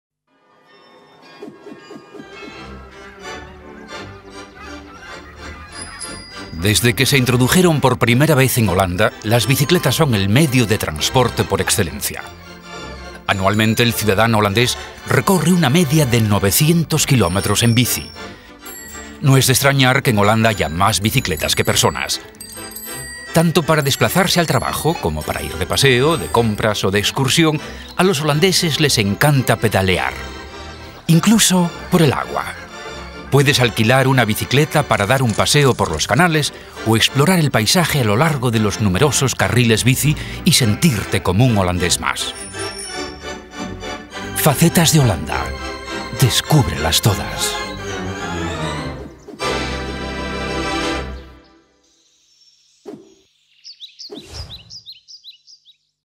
Te presentamos: la bici
Descubre Holanda en una bici de caballero, en una bici de mamá, en la bici de paseo de la abuela, en un tándem, en una bici de carreras, en una bici horizontal, en una bici de transporte, en un triciclo, en una bici plegable o en una bici eléctrica. Pedalea a lo largo de los canales de Ámsterdam, recorre la costa holandesa, atraviesa los coloridos campos de bulbos florales, impúlsate por las praderas salpicadas de vacas y molinos de viento, sube las verdes colinas del sur, piérdete por los bosques pasando junto a los castillos más hermosos del este o realiza una caminata por los alrededores del lago IJssel (IJsselmeer) o una de las islas del mar de Wadden.